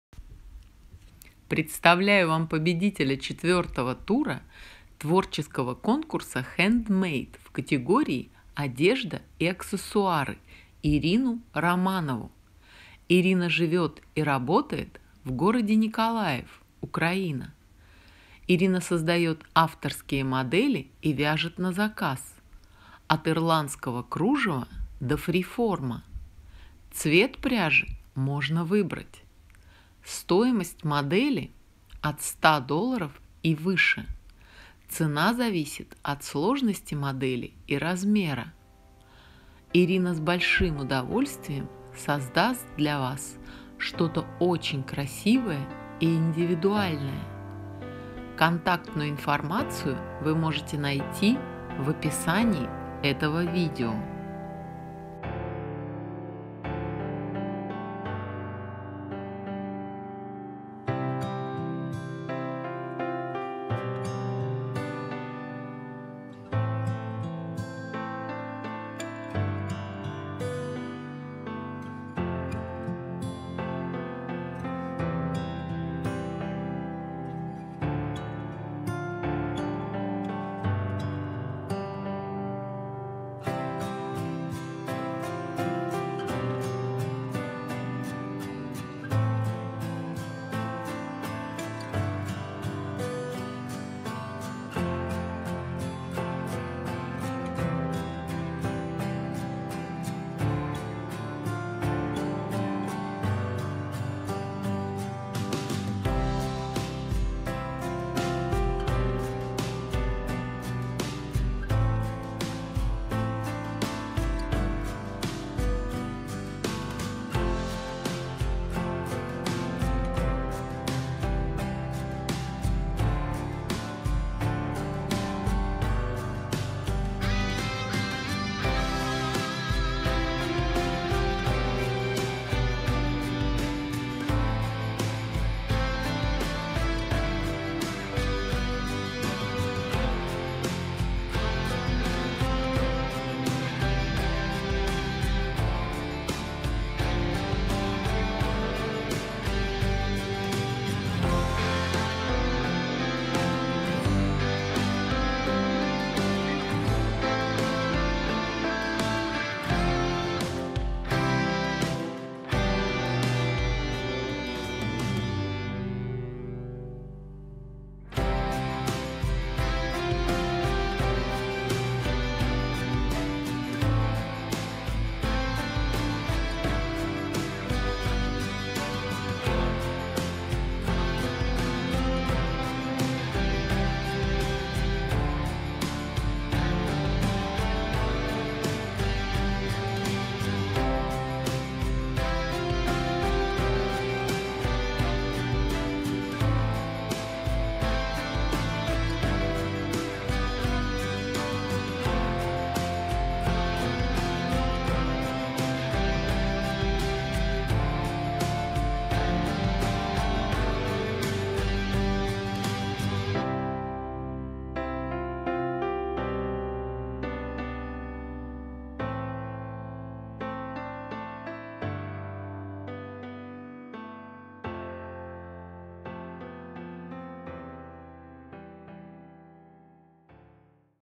Ирина Романова. Вязание. Авторские работы.
Представляю вам победителя 4 тура творческого конкурса "Hand made"  в категории "Одежда и аксессуары" - Ирину Романову!
Ирина живет и работает в городе Николаев, Украина.

Ирина создает авторские модели и вяжет на заказ -  от ирландского кружева до фриформа. Цвет пряжи можно выбрать. Стоимость модели от 100$ и выше. Цена зависит от сложности модели и размера.

Ирина с большим удовольствием создаст для Вас что-то очень красивое и индивидуальное!